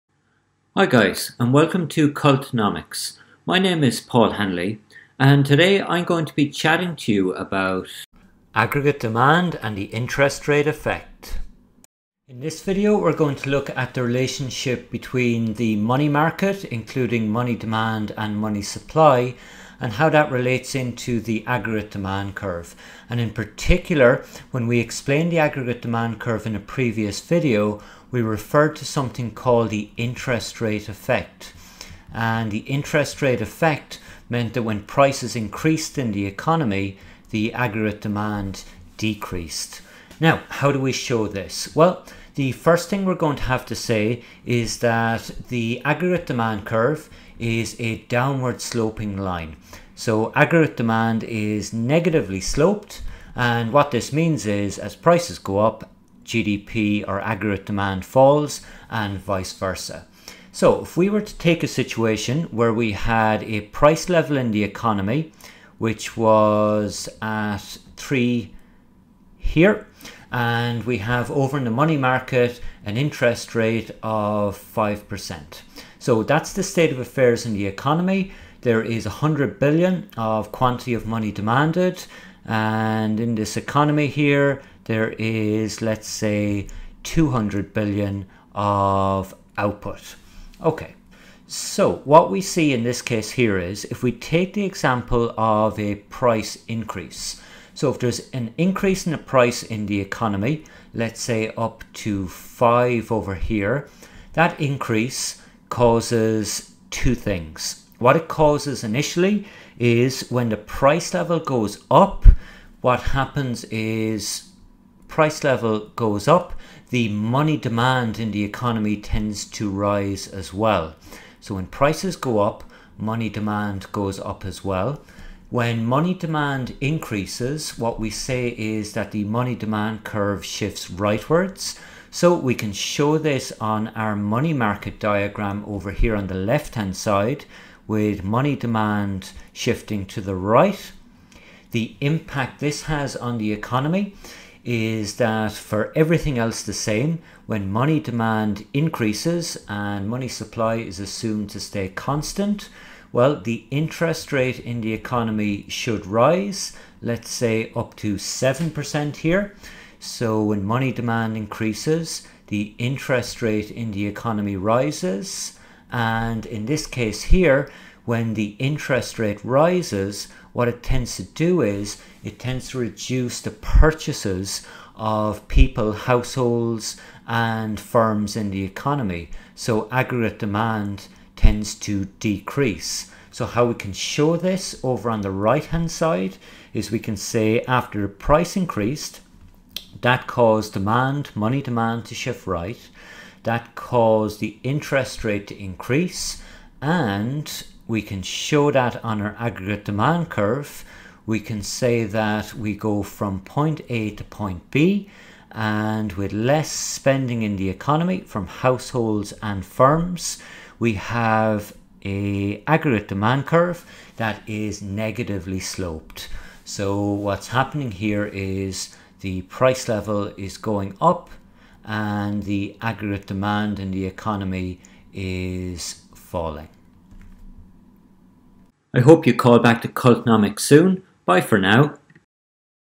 AD and the interest rate effect
This video uses a diagram to explain the interest rate effect in relation to the aggregate demand curve.